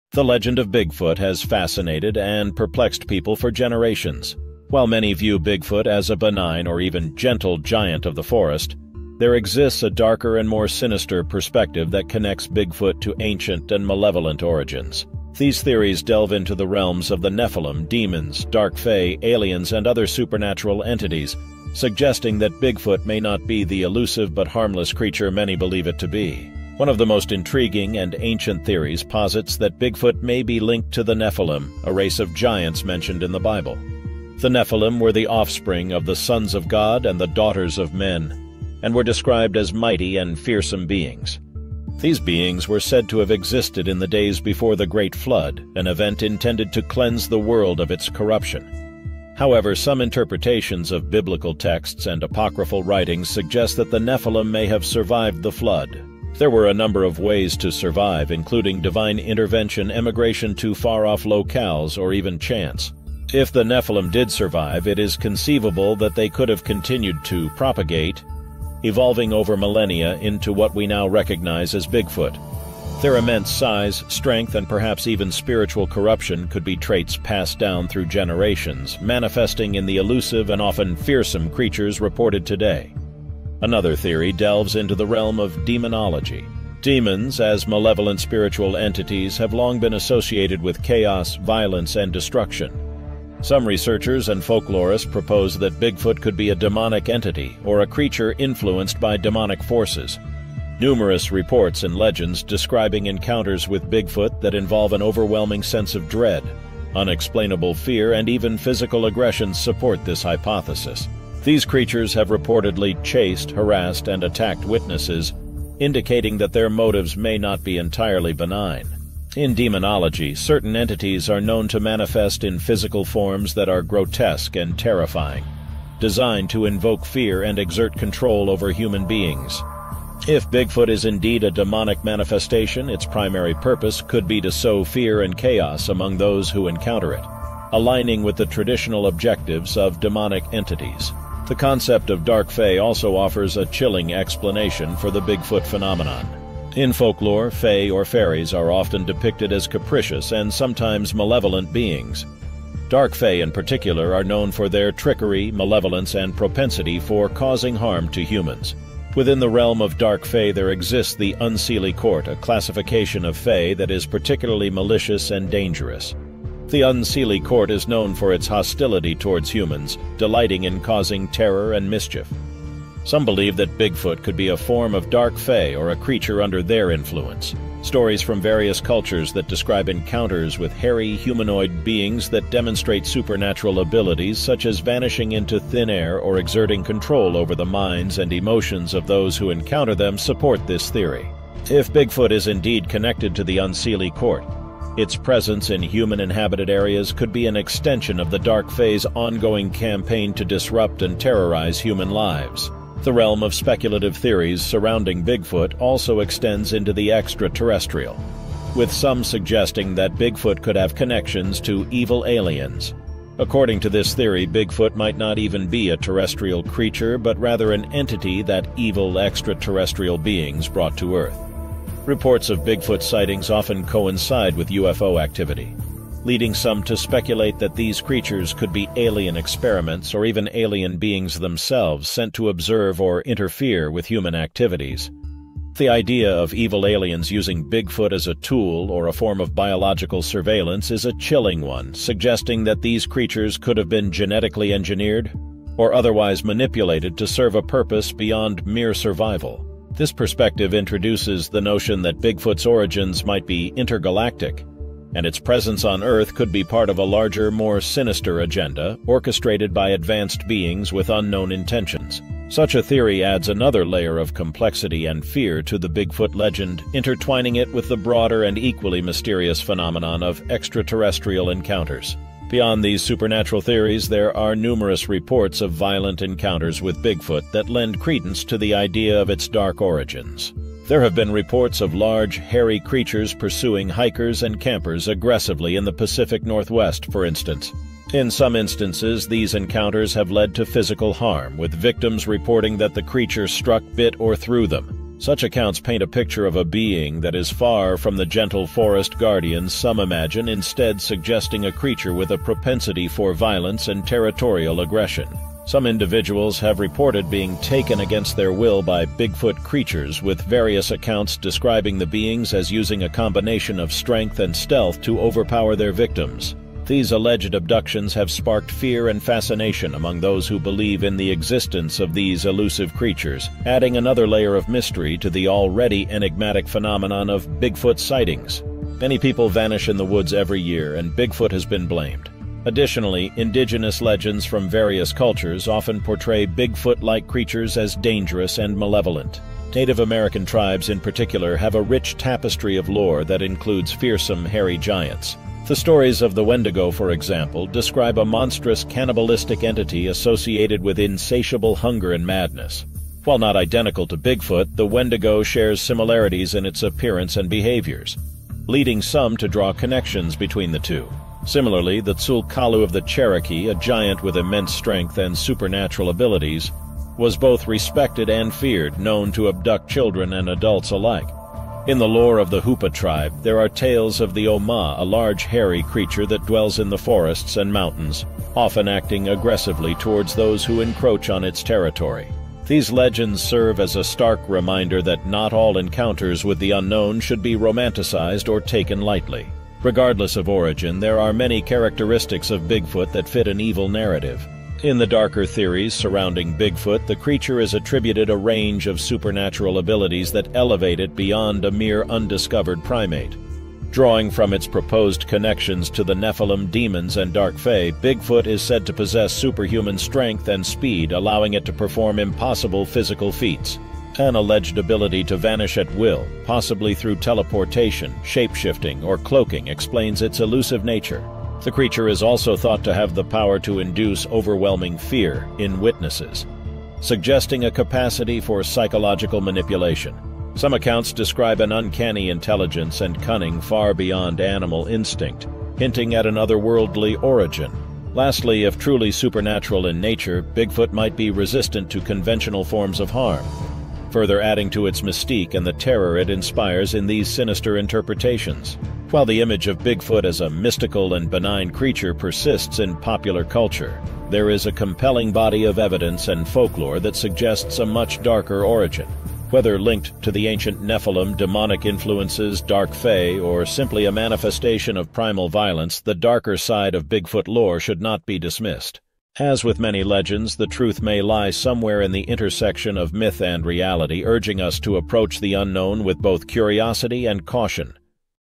Dark Bigfoot: Evil Origins
Uncover the chilling truth behind the Bigfoot legend in our latest video. Forget everything you thought you knew about this elusive creature. We're diving deep into the sinister theories that link Sasquatch to ancient, malevolent entities.

From biblical giants to demonic manifestations, we explore the unsettling connections between Bigfoot and supernatural forces. Could this cryptid be a descendant of the mighty Nephilim? Or perhaps a shape-shifting demon stalking our forests? We weigh in on these spine-tingling possibilities.

But that's not all. We'll also delve into the terrifying world of violent Bigfoot encounters. Prepare yourself for heart-pounding tales of aggression, abduction, and unexplained disappearances. These aren't your typical Bigfoot sightings – these are stories that will make you think twice about your next camping trip.

Don't miss this eye-opening journey into the darker side of cryptozoology. The truth is out there, and it's more frightening than you ever imagined.